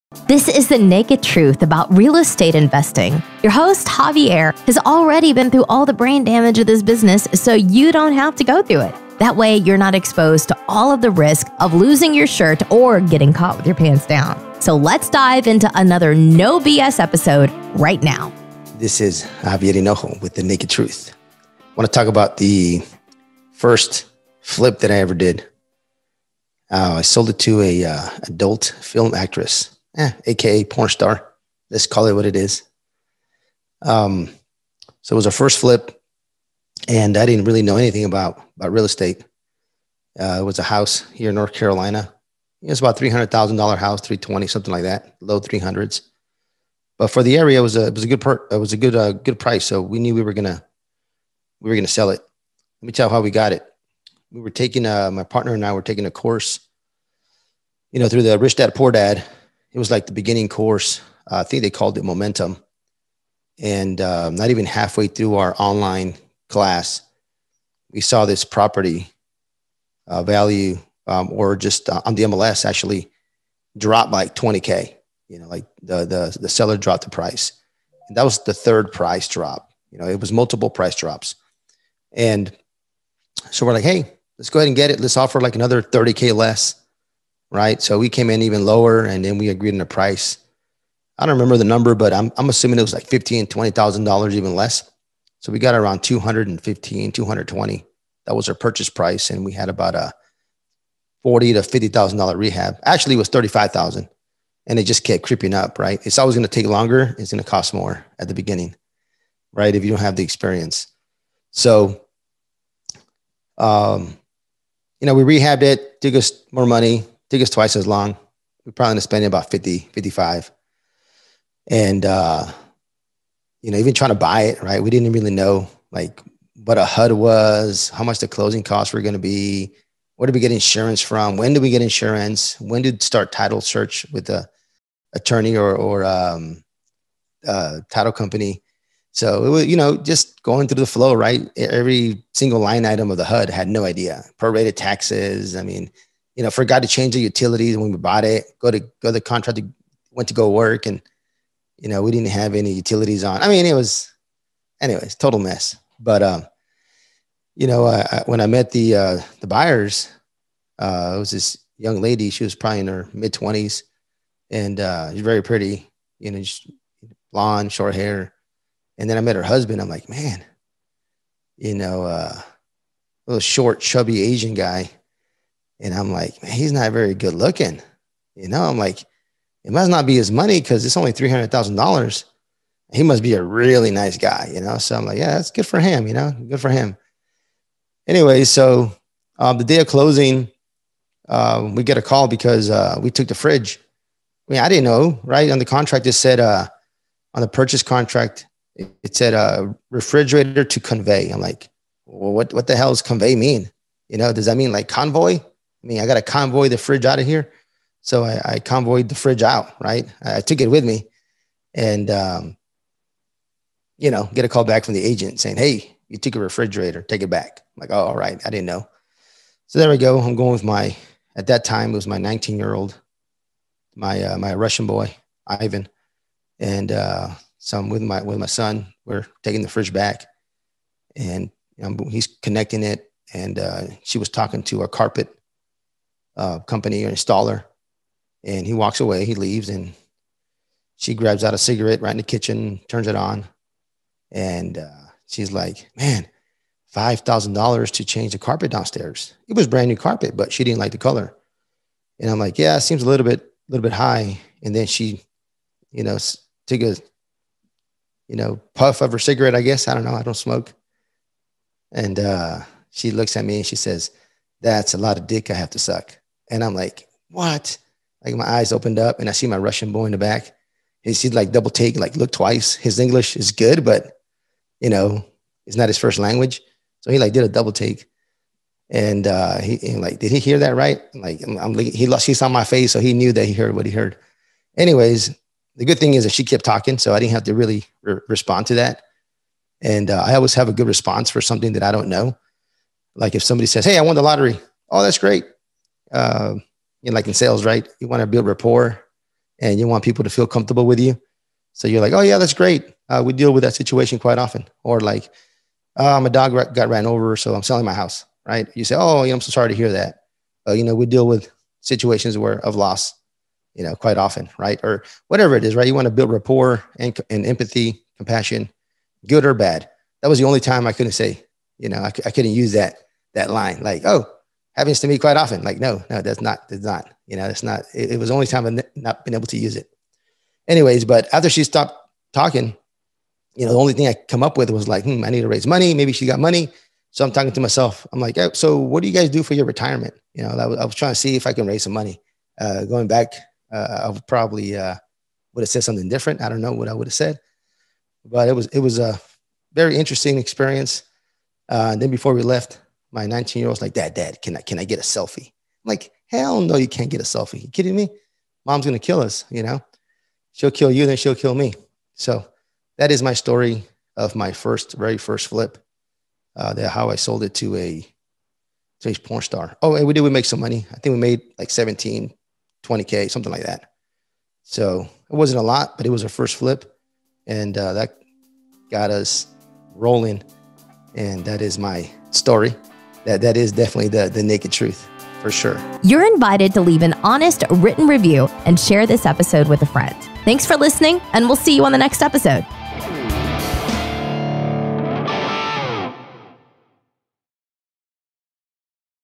The Naked Truth Ep 01: I Sold My First Flip To A Pornstar
Tune in to today's episode as Javier Hinojo discusses selling his first flip to a client he didn't know at the time was famous and made a shocking statement that surprised him.

In this episode, Javier tells the naked truth about selling his first flip and the most interesting experience of meeting his first buyer. Tune in for more episodes!

Key takeaways to listen for
• Checking on the first property to flip
• Rehabbing a property to make money
• Meeting the buyers for the first time
• Interesting experience in closing a deal

To learn more about partnering with us, visit our